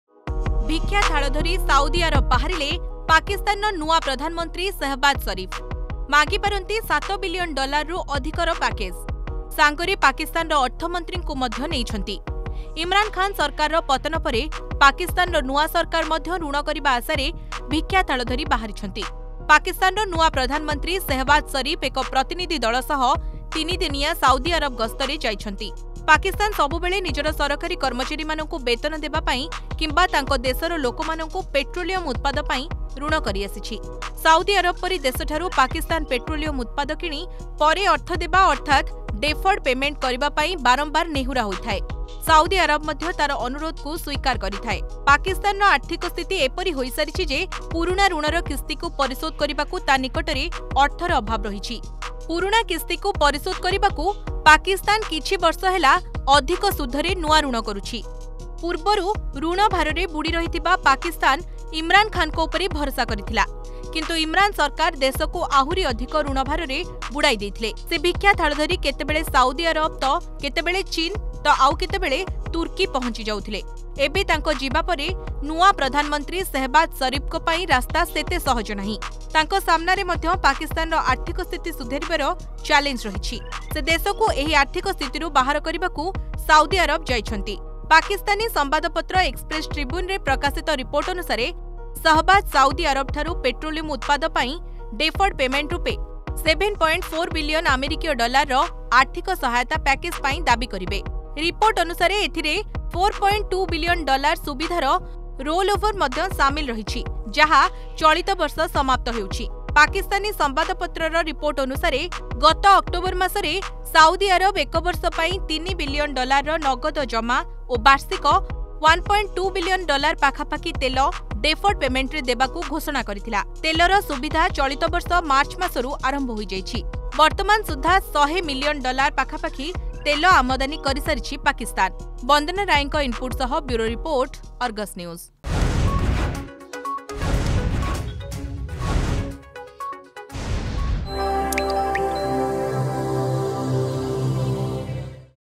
ପୁଣି ଥରେ ଭିକ୍ଷା ଥାଳ ଧରି ଗସ୍ତ
ଭିକ୍ଷା ଥାଳ ଧରି ସାଉଦି ଆରବ ବାହାରିଲେ ପାକିସ୍ତାନର ନୂଆ ପ୍ରଧାନମନ୍ତ୍ରୀ ଶହବାଜ ଶରିଫ । ମାଗି ପାରନ୍ତି ସାତ ବିଲିୟନ ଡଲାରରୁ ଅଧିକର ପ୍ୟାକେଜ । ସାଙ୍ଗରେ ପାକିସ୍ତାନର ଅର୍ଥ ମନ୍ତ୍ରୀଙ୍କୁ ମଧ୍ୟ ନେଇଛନ୍ତି ।